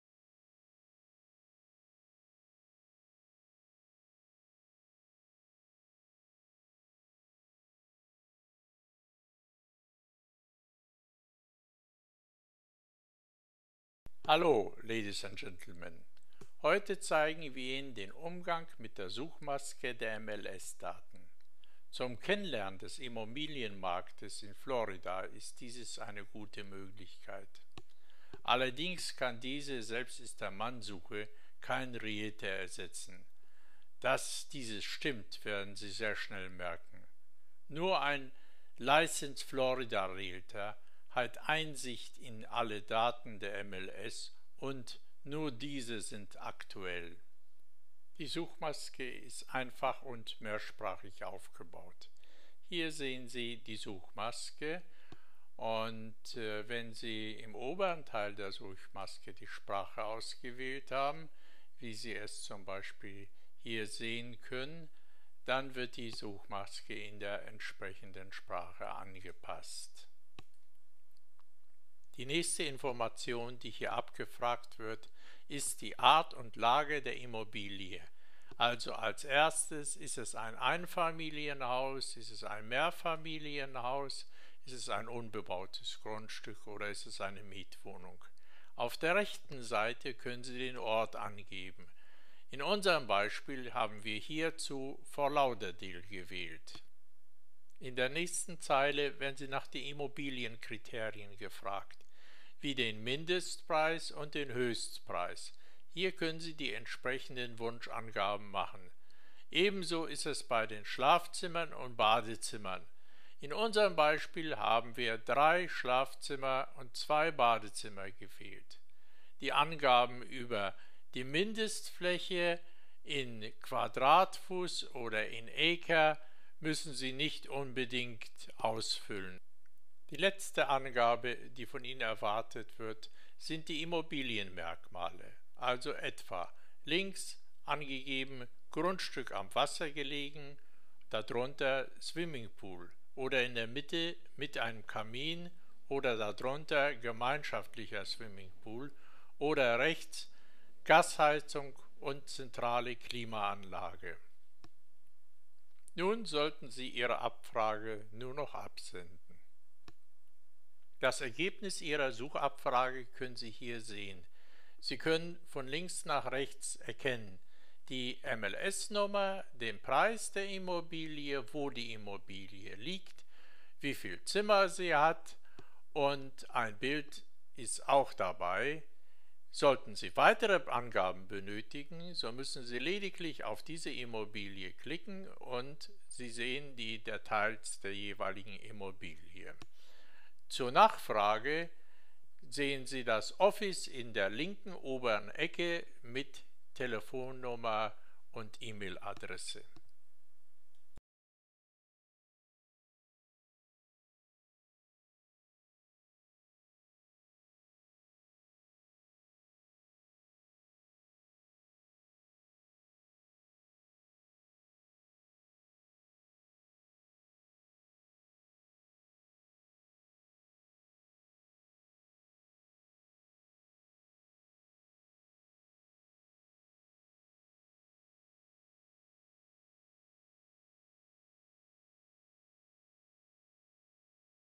Umgang mit dem auf unserer Homesuite fuer Sie bereitgestellten Zugang zu den MLS Daten und der Abfra
Warum also ein Realtor ? Weil das nicht alles ist. Die eigentliche Arbeit beginnt erst jetzt. Fehler, die Sie ab hier machen, koennen fuer Sie teuer werden. Ein Realtor kostet Sie nichts.